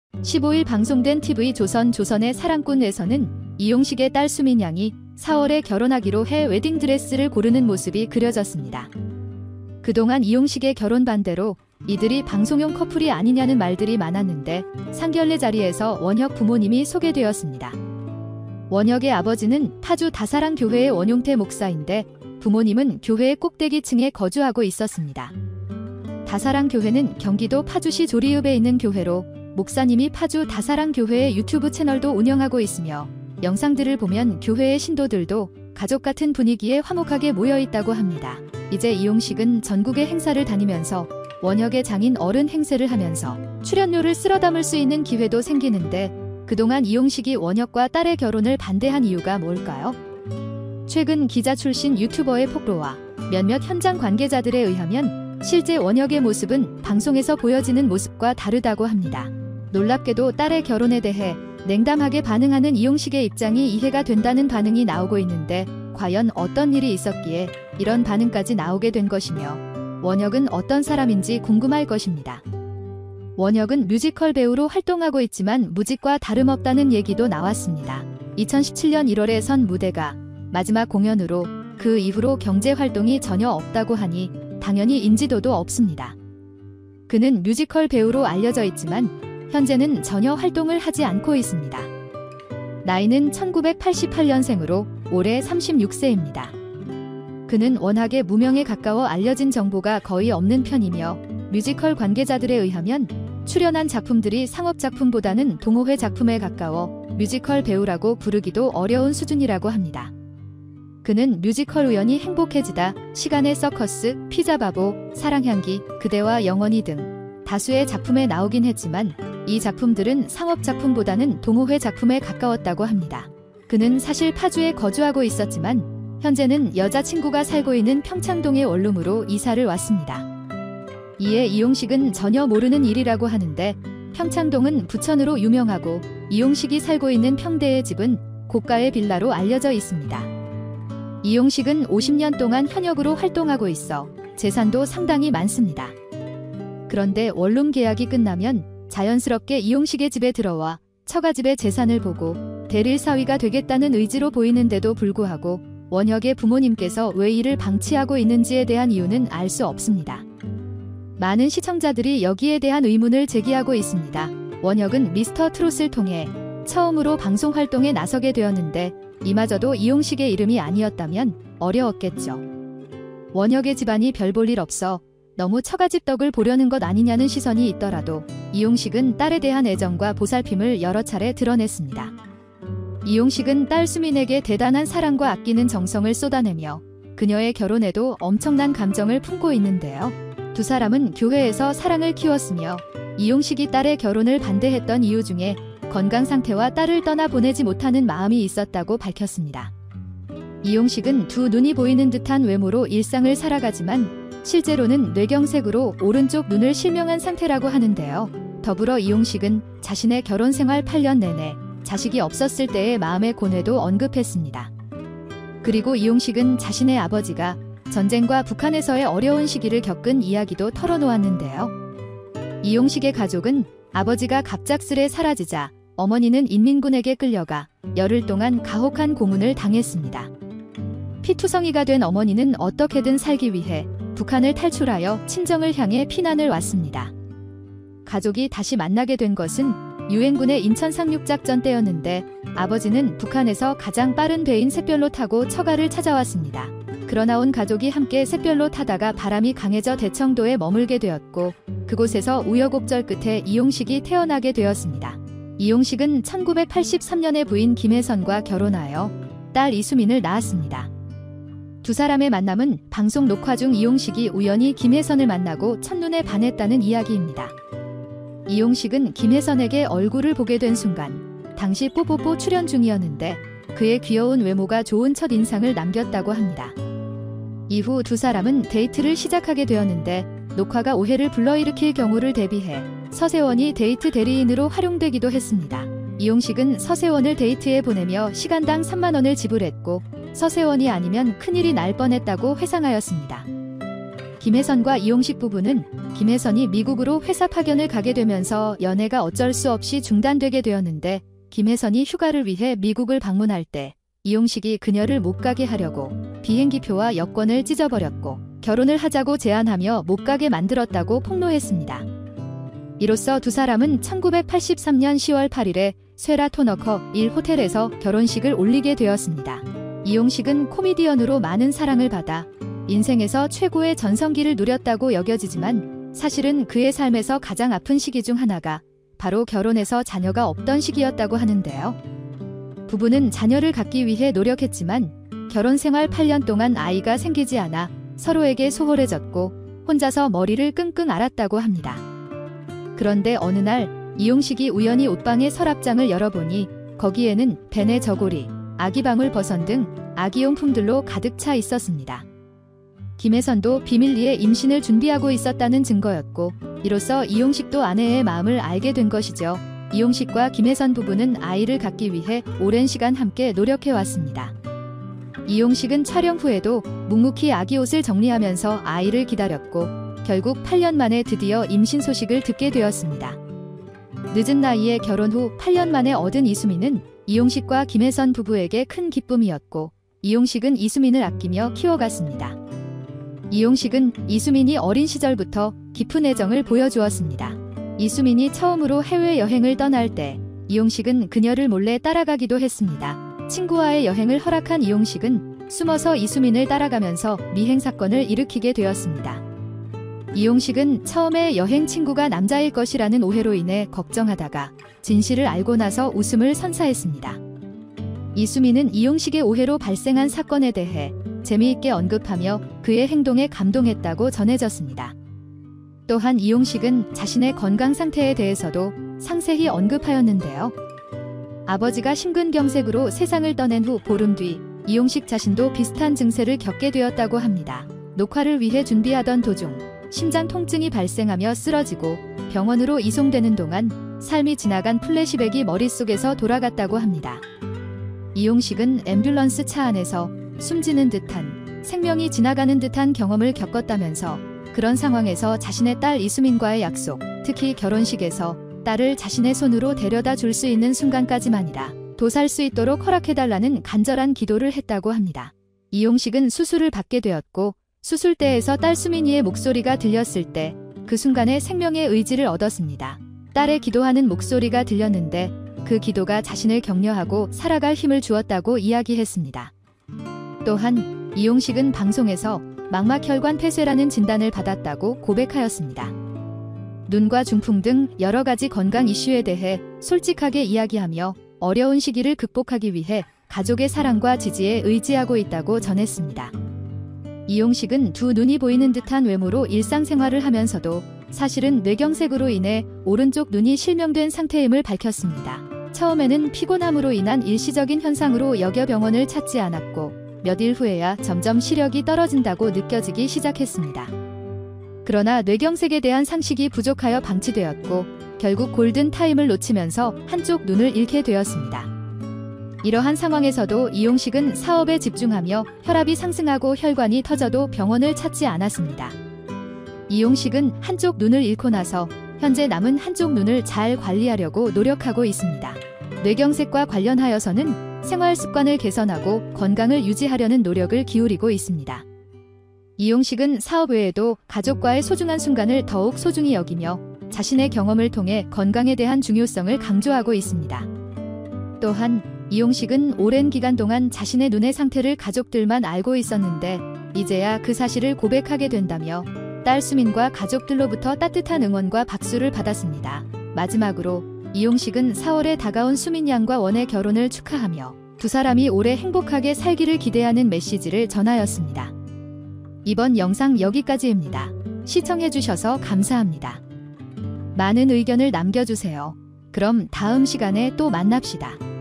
이용식 딸 예비남편 부모 상견례 충격! 이용식 반대한 사위 원혁 아버지 알고보니?!
故최진실 딸 최준희 최근에 할머니한테 했던 망행들 결국 분노한 오빠 최환희 충격적 고백..